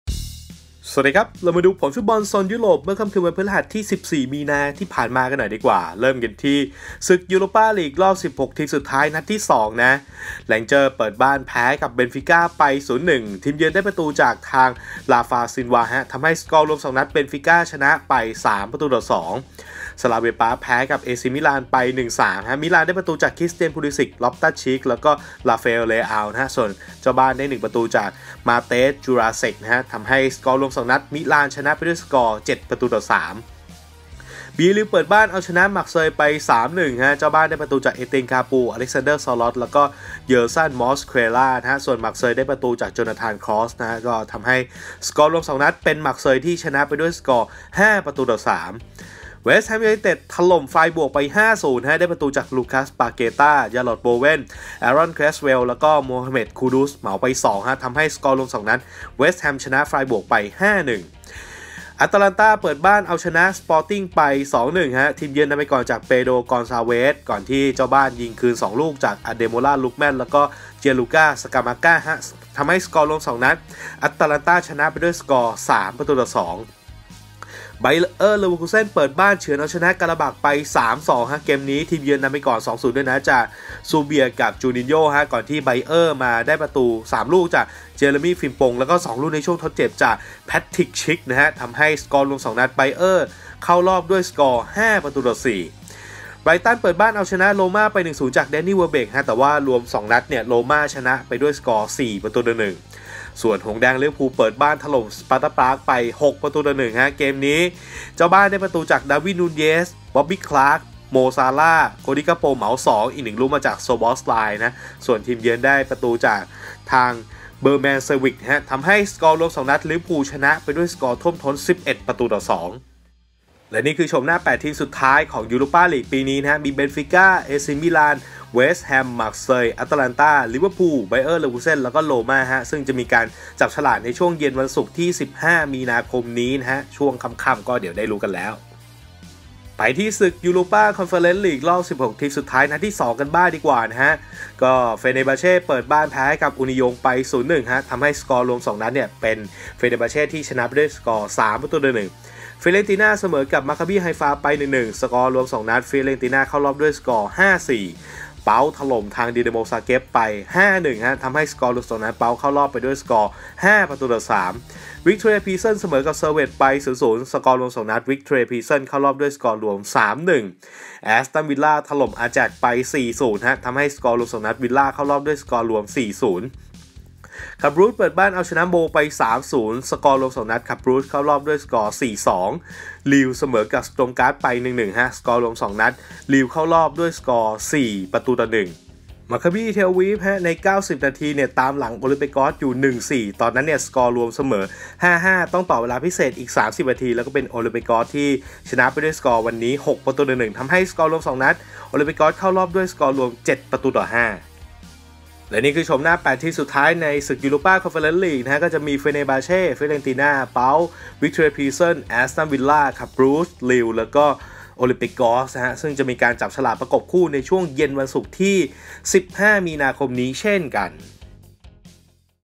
ผลบอลเมื่อคืนโซนยุโรป : UEL รอบ 16 ทีม นัด 2 | UECL รอบ 16 ทีม นัด 2 (14/3/24)
Timestamp

00:00 UEL รอบ 16 ทีม นัด 2
02:20 UECL รอบ 16 ทีม นัด 2